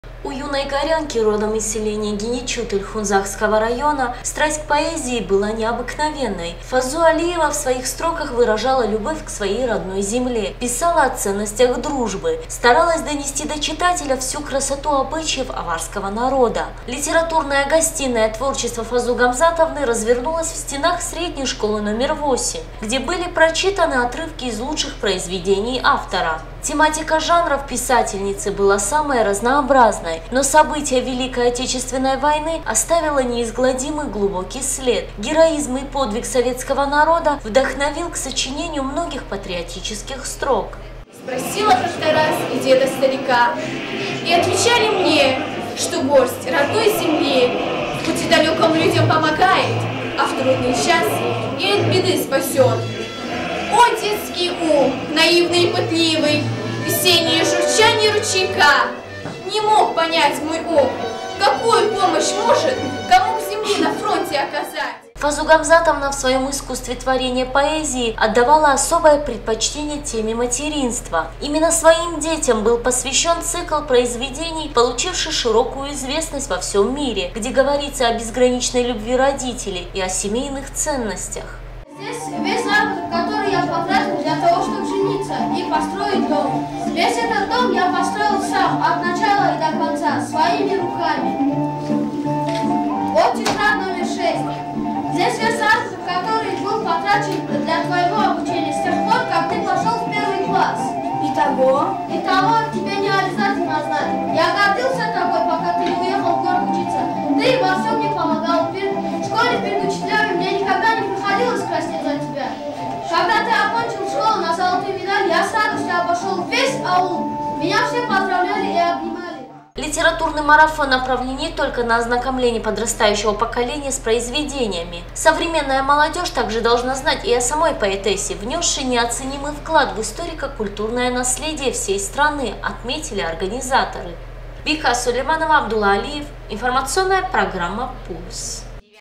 Фазу Алиева голос мира и добра Литературный марафон в школе №8
В общеобразовательных учреждениях Хасавюрта продолжается литературный марафон посвященный творчеству Фазу Гамзатовны Алиевой, организованный по инициативе руководства центральной библиотеки имени Расула Гамзатова. В средней школе №8 прошел творческий вечер, где участникам представилась возможность узнать известные произведения поэтессы, получившие мировое признание.